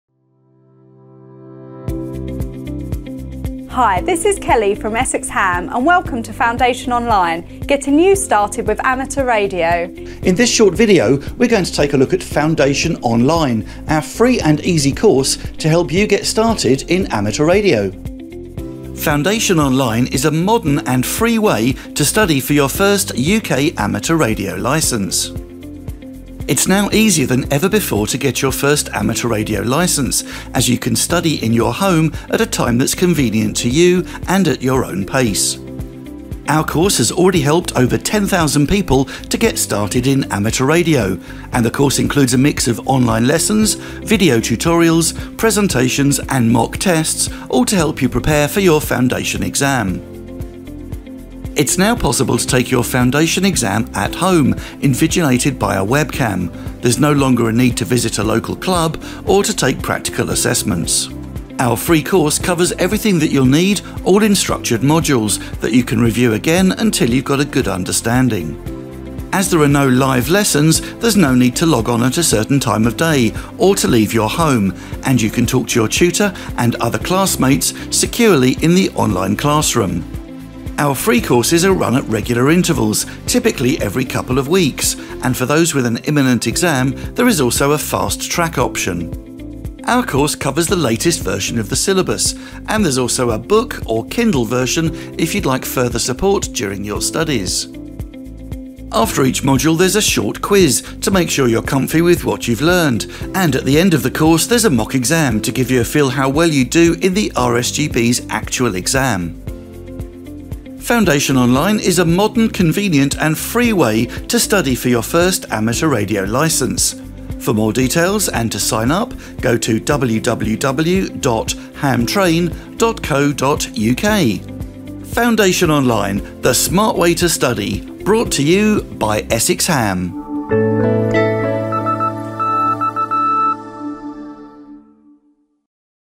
Foundation Online Amateur Radio Training Course
Essex Ham runs "Foundation Online "- a free online amateur radio Foundation training course, aimed at helping more people to take up the hobby. We've helped over 11,000 to get started.

This short video demonstrates how the distance-learning course works, and how to study online to get an amateur radio licence.